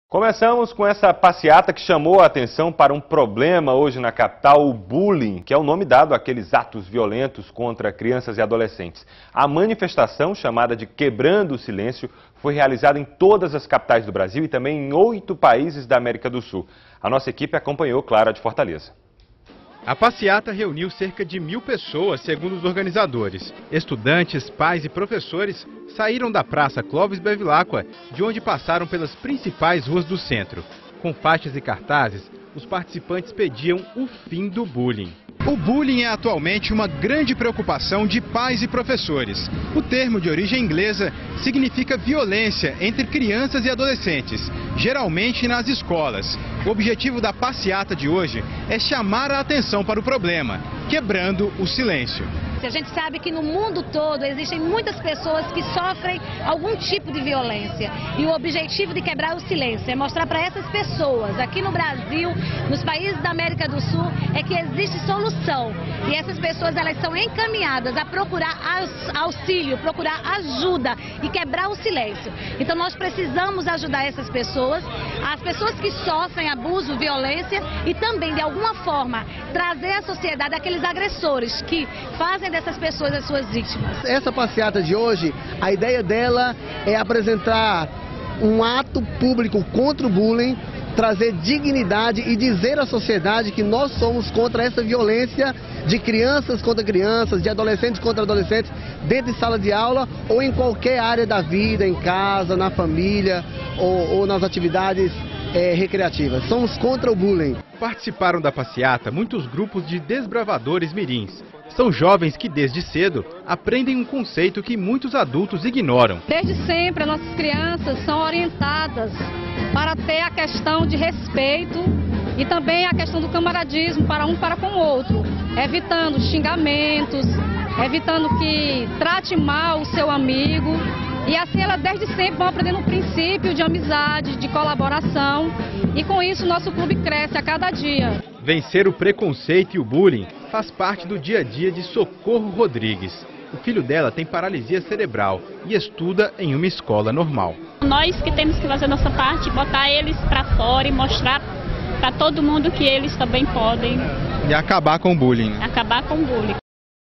Quebrando o Silêncio - Rede Globo Ceará
Sábado, 27/08/2011 - Pais e alunos de escolas de Fortaleza fazem passeata contra Bullying. A ação, segundo organizadores, também aconteceu em outros estados brasileiros.